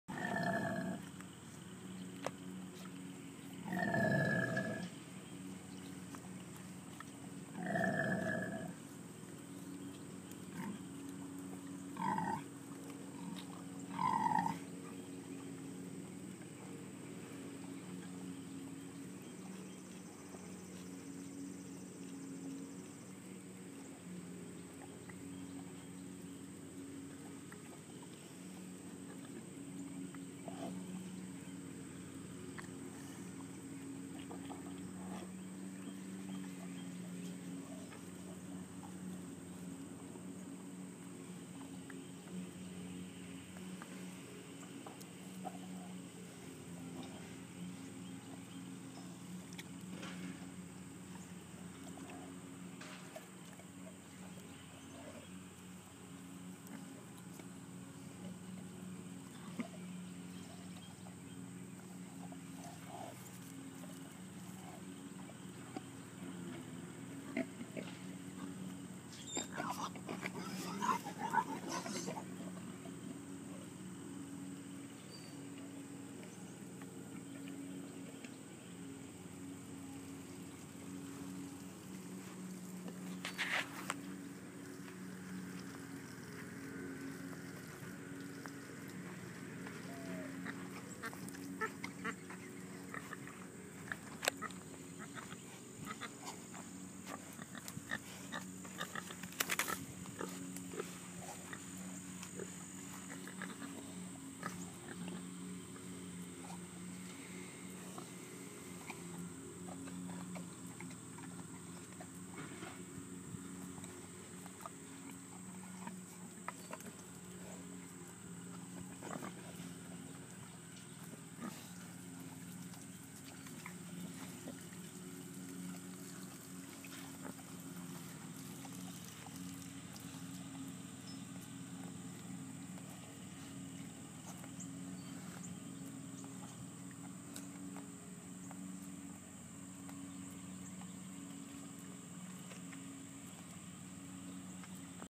Collins Kids Adventures - Linda and Lou's first boar friend
Linda and Lou interacting with their first boar friend